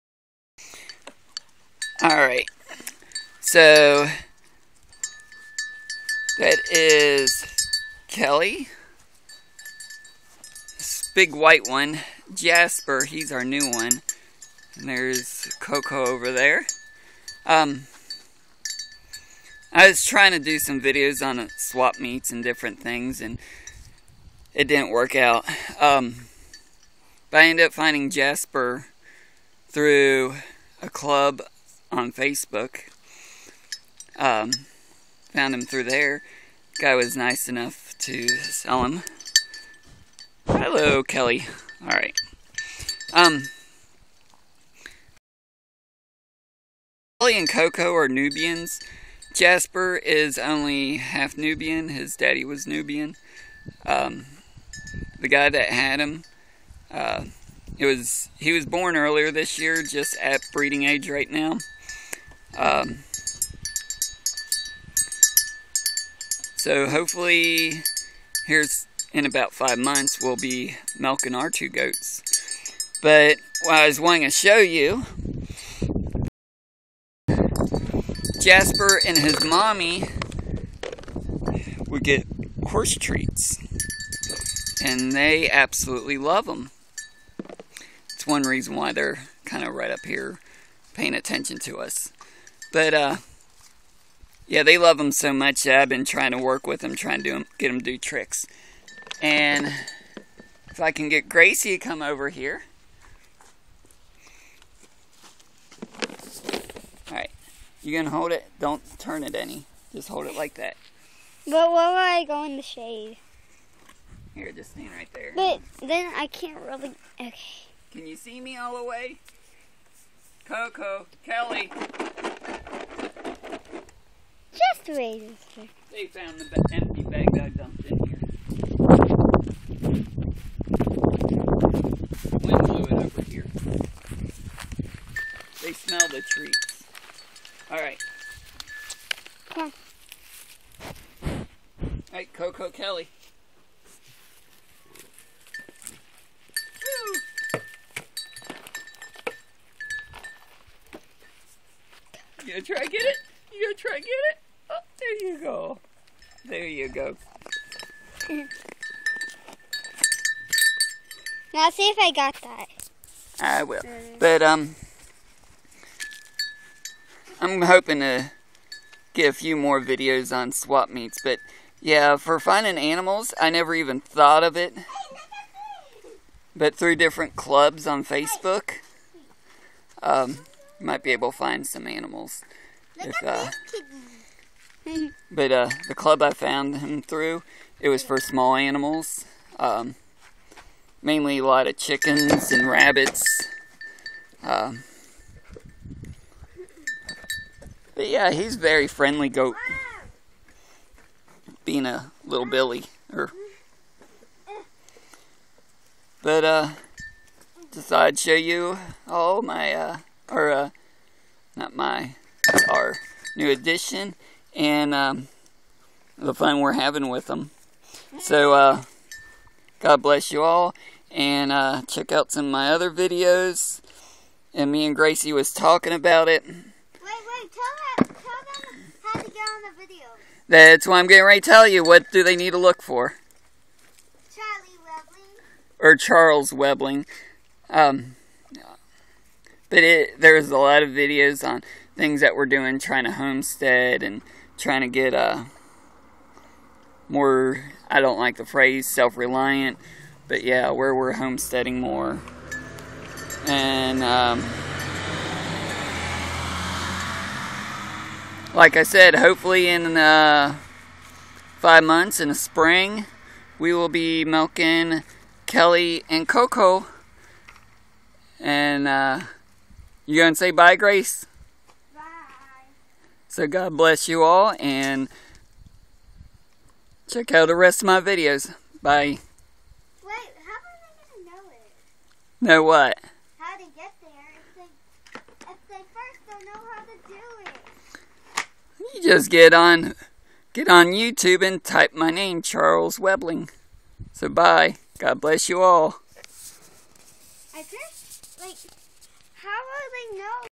Our new goat and their tricks
Showing off Our new goat. The trick we are working on.